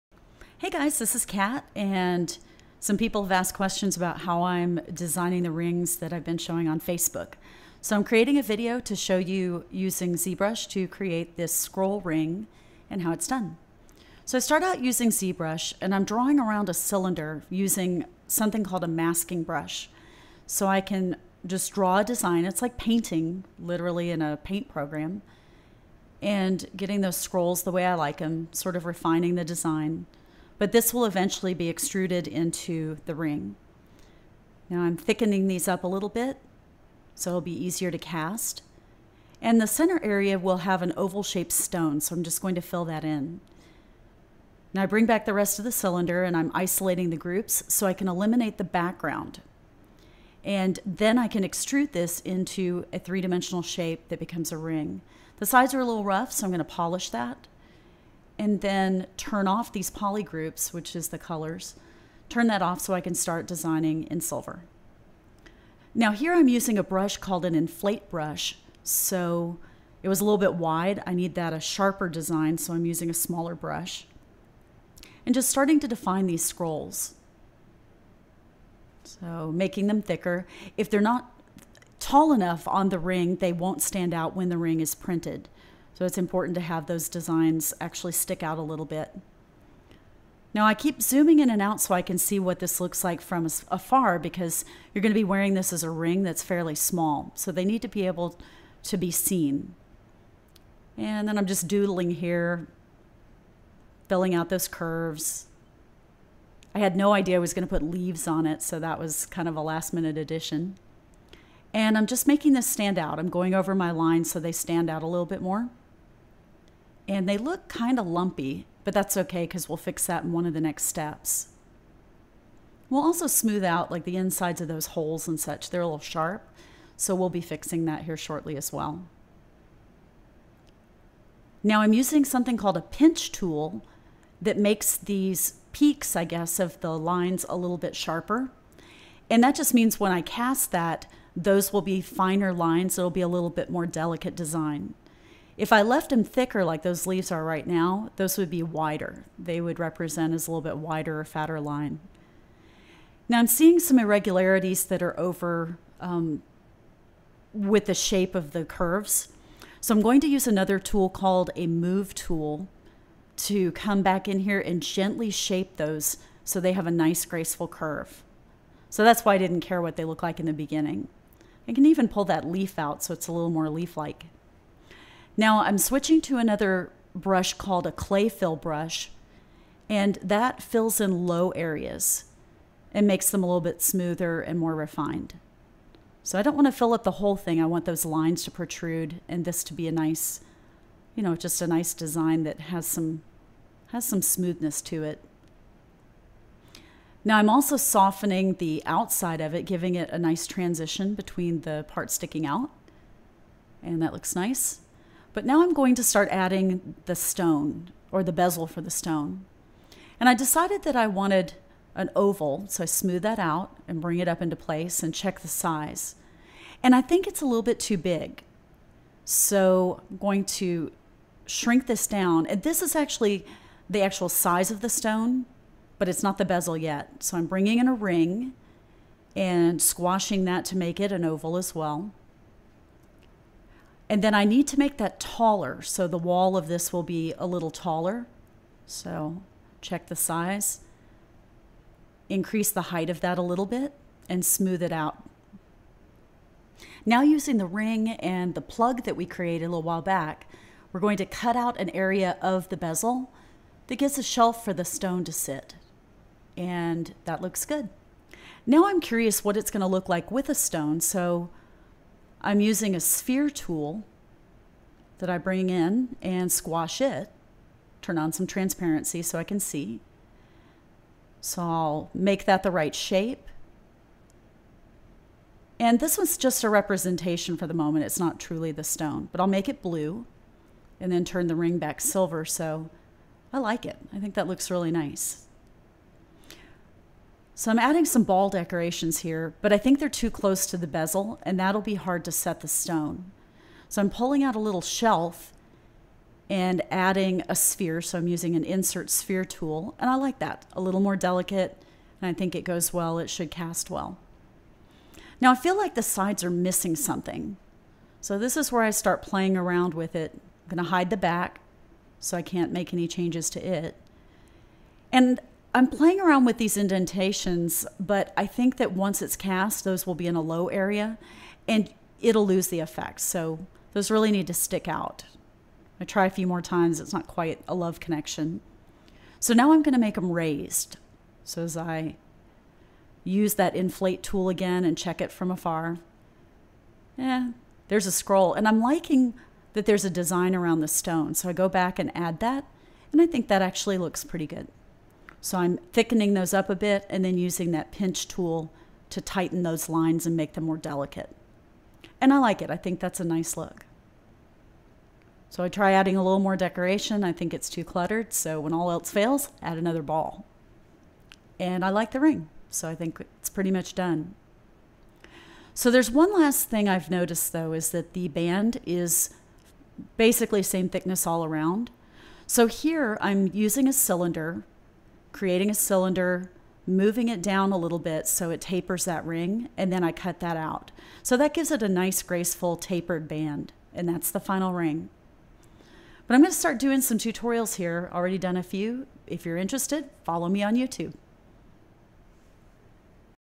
Scroll Ring in ZBrush Demo
This tutorial shows a technique for creating a scroll ring in ZBrush.  One thing to note...this design is not ideal for actual casting...when the design protrudes a little more, it works better.